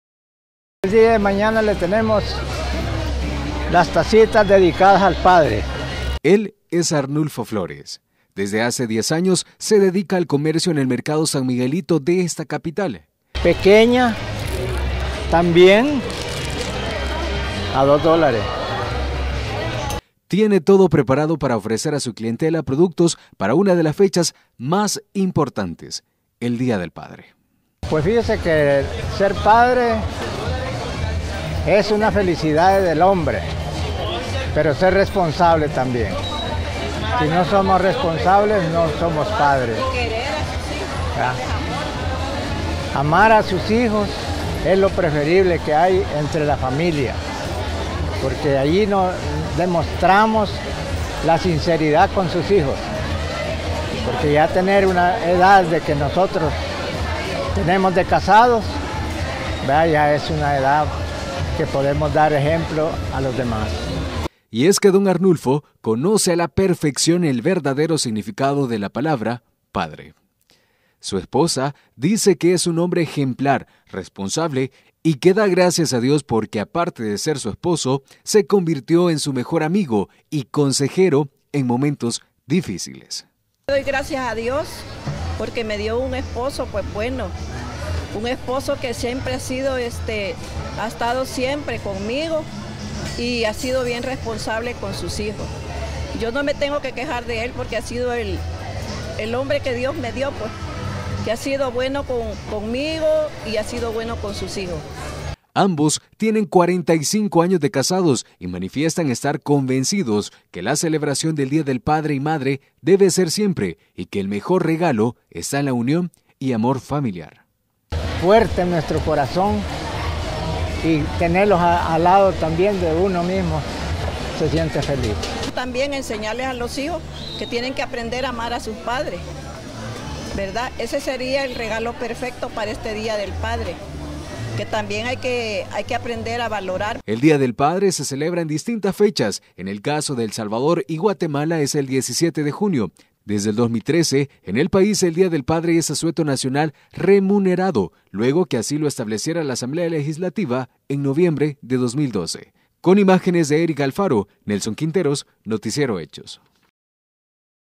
Historias del Día del Padre
En el mercado San Miguelito encontramos un  matrimonio de 45 años quienes nos cuentan cual es el principal regalo que se debe dar en este día especial.